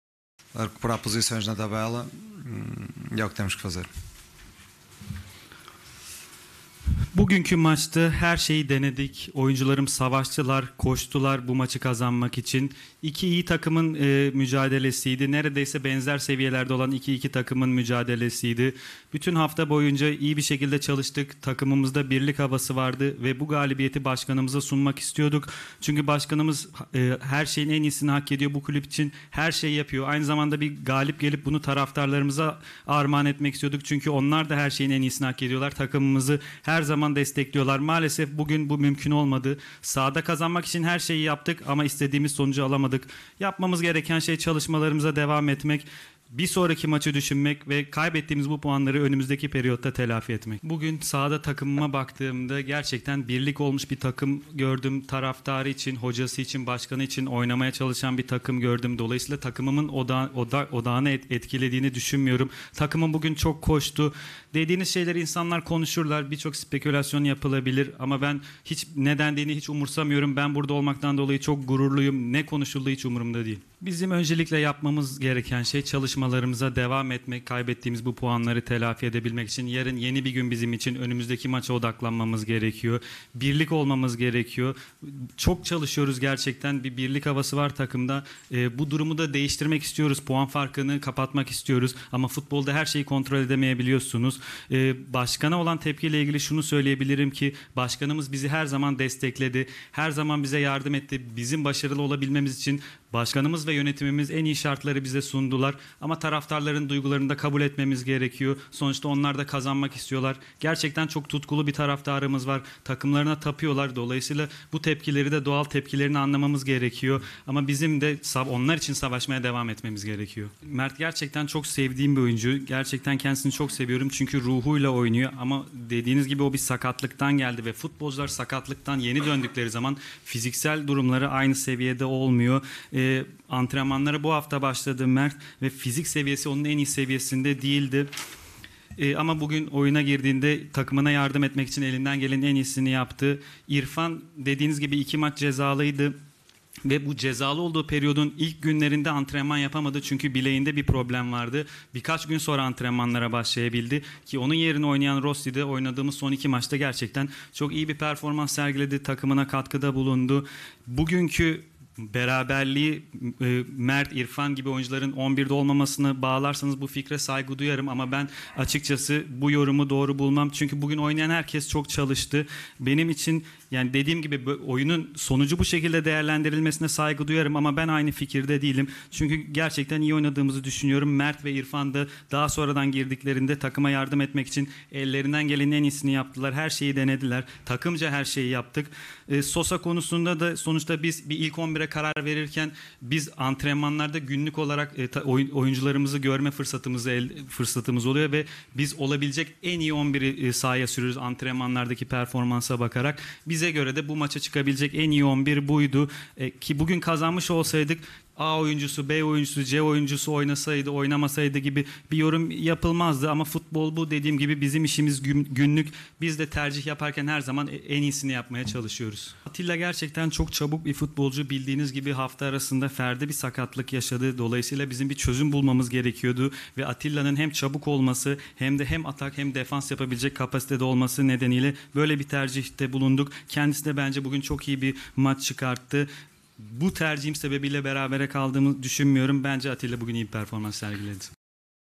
Vitor Pereira: "Takımıma Baktığımda Birlik Olduğunu Gördüm!" (Fenerbahçe 2-2 Beşiktaş) / 20.12.2021
Fenerbahçe, Süper Lig'in 17. haftasında Beşiktaş ile evinde 2-2 berabere kaldı. Maçtan sonra Teknik Direktör Vitor Pereira basın toplantısı düzenledi. Pereira, "İki iyi takım karşılaştı. Başkanımıza galibiyet hediye etmek istiyorduk ama olmadı. Takımıma baktığımda birlik olduğunu gördüm. Ben burada olmaktan gururluyum, çıkan spekülasyonlar umurumda değil. Başkana olan tepki hakkında şunu diyebilirim. Başkanımız bize çok destek veriyor ama taraftarların duygularını kabul etmemiz gerekiyor. " dedi..